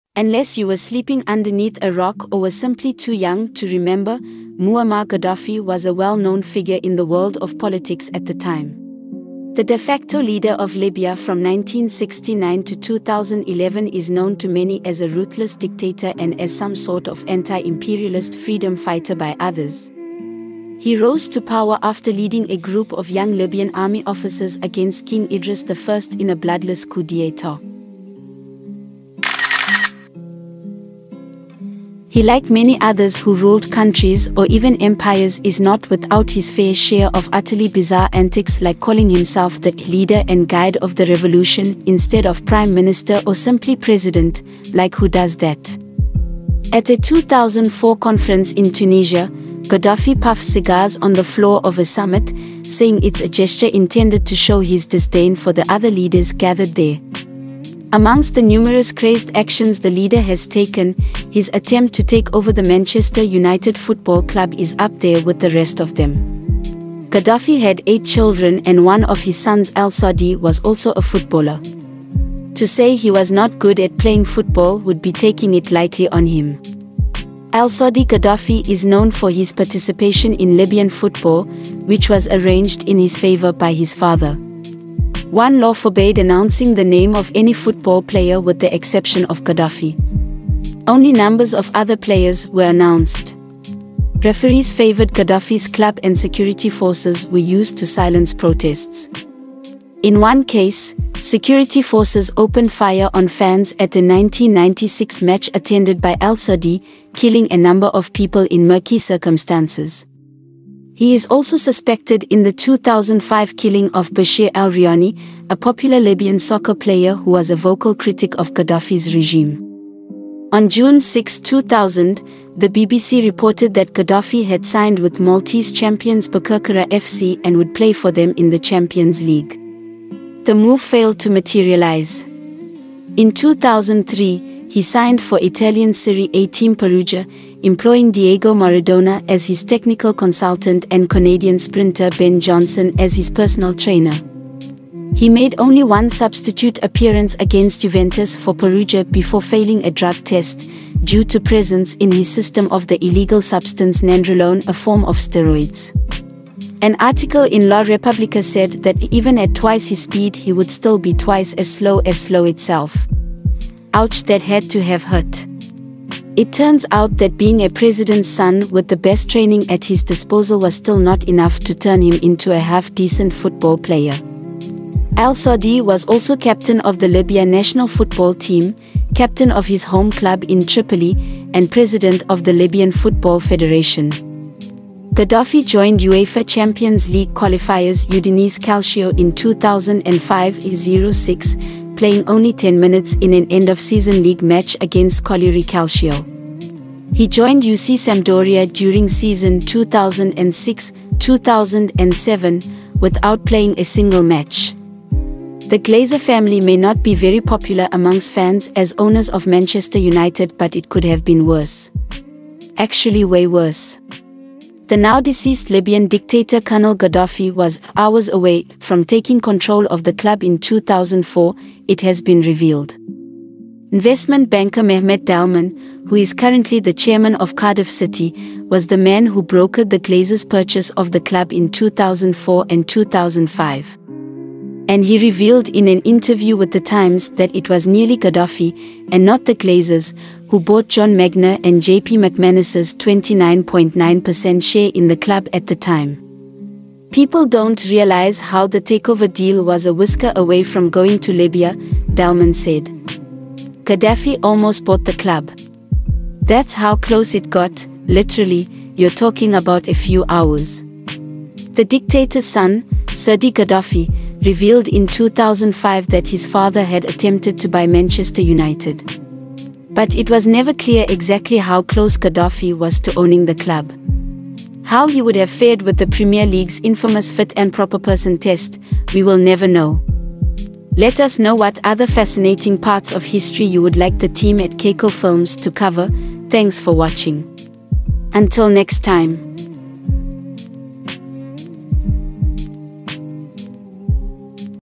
The Shocking Facts: How Muammar Gaddafi Almost Manchester United Football Club
In this video, we reveal the little-known story of how the late Libyan dictator, Muammar Gaddafi, came close to buying a significant stake in one of the world's most popular football clubs, Manchester United. Using exclusive insider information and expert analysis, we explore the backstory of the potential deal, including Gaddafi's motivations, the negotiations with United's ownership, and the reasons why the deal ultimately fell through. We also delve into the impact that this near-acquisition could have had on both the football club and the geopolitical landscape of the time. If you're a football fan, a history buff, or just love uncovering hidden secrets, this is a must-watch video for you. Don't miss out on this fascinating and informative deep-dive into the intriguing tale of Muammar Gaddafi and Manchester United.